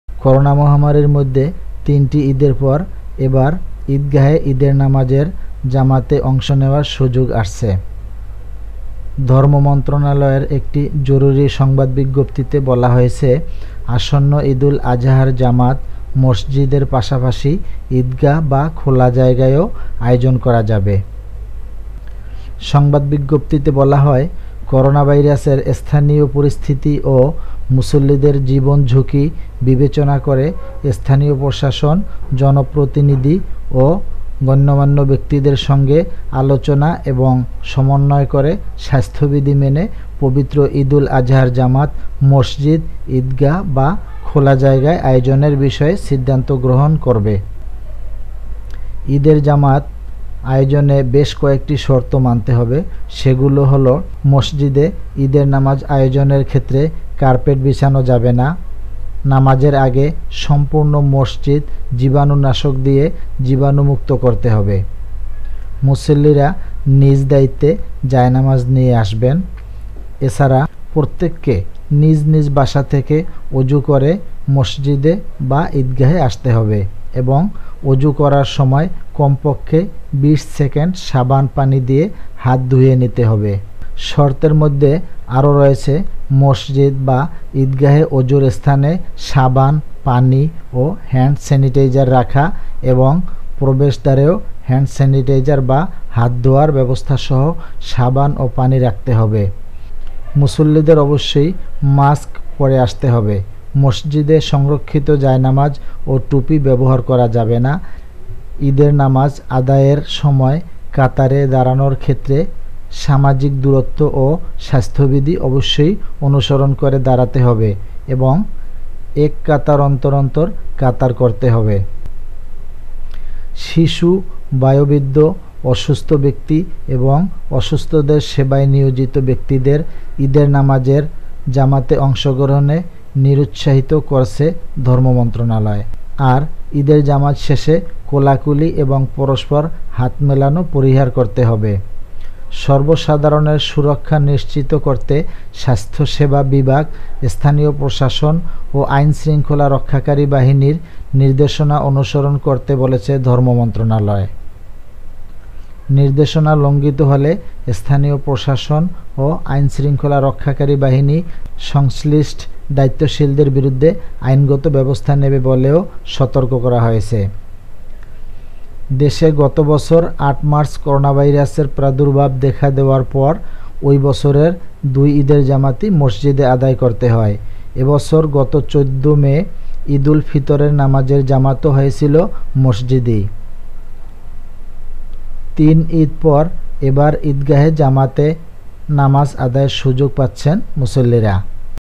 মসজিদ এর পাশাপাশি খোলা মাঠে এবারের ঈদের জামাত।
করোনা মহামারির মধ্যে তিনটি ঈদের পর এবার ঈদগাহে ঈদের নামাজের জামাতে অংশ নেওয়ার সুযোগ আসছে। ধর্ম মন্ত্রণালয়ের একটি জরুরি সংবাদ বিজ্ঞপ্তিতে বলা হয়েছে, আসন্ন ঈদুল আজহার জামাত মসজিদের পাশাপাশি ঈদগাহ বা খোলা জায়গায়ও আয়োজন করা যাবে। সংবাদ বিজ্ঞপ্তিতে বলা হয়, করোনাভাইরাসের স্থানীয় পরিস্থিতি ও মুসল্লিদের জীবন-ঝুঁকি বিবেচনা করে স্থানীয় প্রশাসন জনপ্রতিনিধি ও গণ্যমান্য ব্যক্তিদের সঙ্গে আলোচনা এবং সমন্বয় করে স্বাস্থ্যবিধি মেনে পবিত্র ঈদুল আজহার জামাত মসজিদ, ঈদগাহ বা খোলা জায়গায় আয়োজনের বিষয়ে সিদ্ধান্ত গ্রহণ করবে। ঈদের জামাত আয়োজনে বেশ কয়েকটি শর্ত মানতে হবে। সেগুলো হলো মসজিদে ঈদের নামাজ আয়োজনের ক্ষেত্রে কার্পেট বিছানো যাবে না। নামাজের আগে সম্পূর্ণ মসজিদ জীবাণুনাশক দিয়ে জীবাণুমুক্ত করতে হবে। মুসল্লিরা নিজ দায়িত্বে জায়নামাজ নিয়ে আসবেন। এ ছাড়া প্রত্যেককে নিজ নিজ বাসা থেকে অজু করে মসজিদ বা ঈদগাহে আসতে হবে এবং অজু করার সময় কমপক্ষে ২০ সেকেন্ড সাবান–পানি দিয়ে হাত ধুয়ে নিতে হবে। শর্তের মধ্যে আরও রয়েছে, মসজিদ বা ঈদগাহে অজুর স্থানে সাবান, পানি ও হ্যান্ড স্যানিটাইজার রাখা এবং প্রবেশদ্বারেও হ্যান্ড স্যানিটাইজার বা হাত ধোয়ার ব্যবস্থাসহ সাবান ও পানি রাখতে হবে। মুসল্লিদের অবশ্যই মাস্ক পরে আসতে হবে। মসজিদে সংরক্ষিত জায়নামাজ ও টুপি ব্যবহার করা যাবে না। ঈদের নামাজ আদায়ের সময় কাতারে দাঁড়ানোর ক্ষেত্রে সামাজিক দূরত্ব ও স্বাস্থ্যবিধি অবশ্যই অনুসরণ করে দাঁড়াতে হবে এবং এক কাতার অন্তর অন্তর কাতার করতে হবে। শিশু, বয়োবৃদ্ধ, অসুস্থ ব্যক্তি এবং অসুস্থদের সেবায় নিয়োজিত ব্যক্তিদের ঈদের নামাজের জামাতে অংশগ্রহণে নিরুৎসাহিত করছে ধর্ম মন্ত্রণালয়। আর ঈদের জামাত শেষে কোলাকুলি এবং পরস্পর হাত মেলানো পরিহার করতে হবে। সর্বসাধারণের সুরক্ষা নিশ্চিত করতে স্বাস্থ্যসেবা বিভাগ, স্থানীয় প্রশাসন ও আইনশৃঙ্খলা রক্ষাকারী বাহিনীর নির্দেশনা অনুসরণ করতে বলেছে ধর্ম মন্ত্রণালয়। নির্দেশনা লঙ্ঘিত হলে স্থানীয় প্রশাসন ও আইনশৃঙ্খলা রক্ষাকারী বাহিনী সংশ্লিষ্ট দায়িত্বশীলদের বিরুদ্ধে আইনগত ব্যবস্থা নেবে বলেও সতর্ক করা হয়েছে। দেশে গত বছর ৮ মার্চ করোনাভাইরাসের প্রাদুর্ভাব দেখা দেওয়ার পর ওই বছরের দুই ঈদের জামাতই মসজিদে আদায় করতে হয়। এ বছর গত ১৪ মে ঈদুল ফিতরের নামাজের জামাতও হয়েছিল মসজিদেই। তিন ঈদ পর এবার ঈদগাহে জামাতে নামাজ আদায়ের সুযোগ পাচ্ছেন মুসল্লিরা।